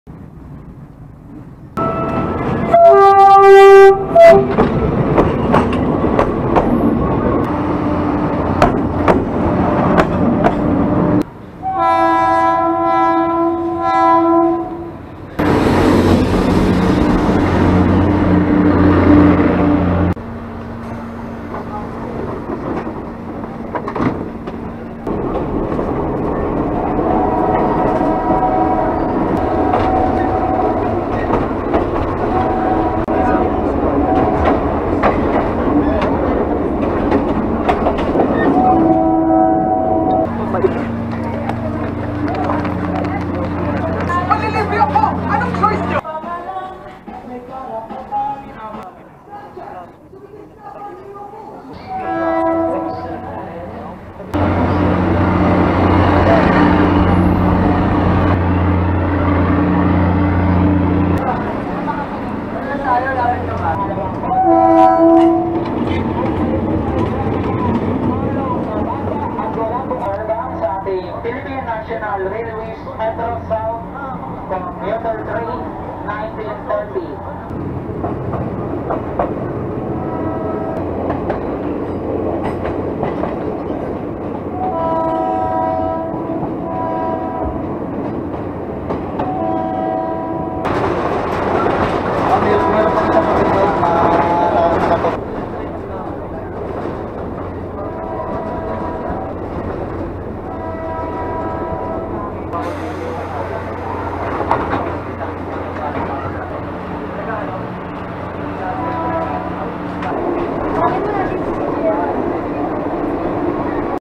The EMU 203 (203系) And The PNR (Philippine National Railways) in HD
Riding the former JNR/JR East 203 series EMU now under PNR in its Metro South Commuter (MSC) service. The train I rode going to (MSC705) was coincidentally the same train I rode going back. Lucky for me, I made it in time for the last trip of the day (MSC1930). Featuring Set 2 hauled by DEL916 (GE U14C) with Set 4 hauled by DEL2535 as an extra. This just covers a short length of the whole trip, though. Hope you enjoy!

Taken with SJCAM 4000 WiFi. First time to test this for action videos (like this one).